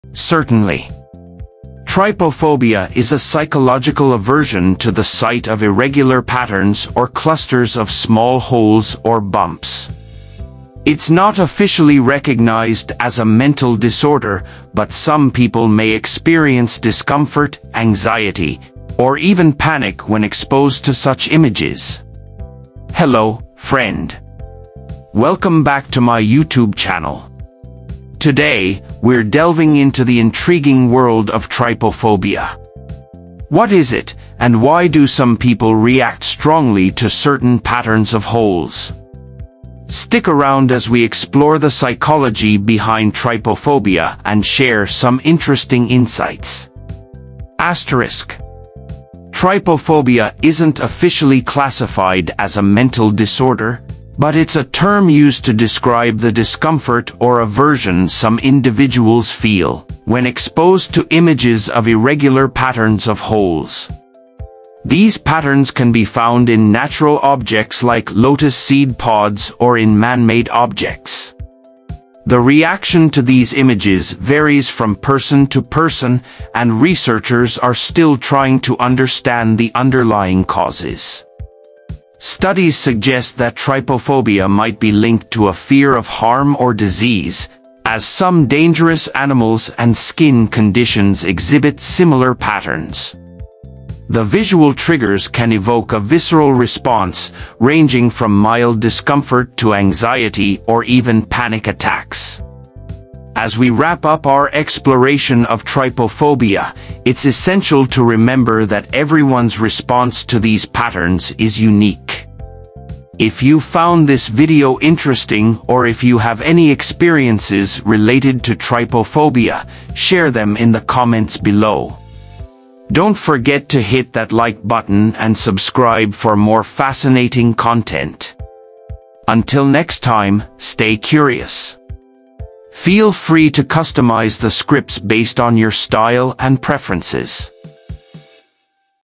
"Unraveling the Mysteries of Trypophobia: A Comprehensive Exploration of the Phenomenon"
"Dive into the intriguing world of Trypophobia with our comprehensive guide, where we unravel the mysteries surrounding this unique psychological phenomenon. Explore the origins, common triggers, and scientific insights behind the aversion to clustered patterns. Gain a deeper understanding of the emotional and physiological responses associated with Trypophobia, shedding light on this enigmatic aspect of human perception."